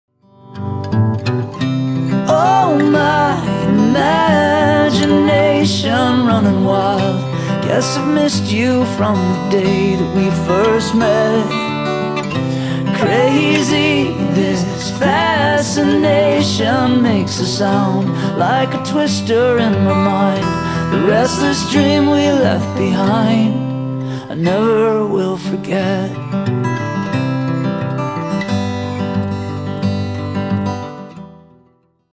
Restless Dream Animated Poster
Part of my Independent Study project in which I designed several music inspired posters and then animated them.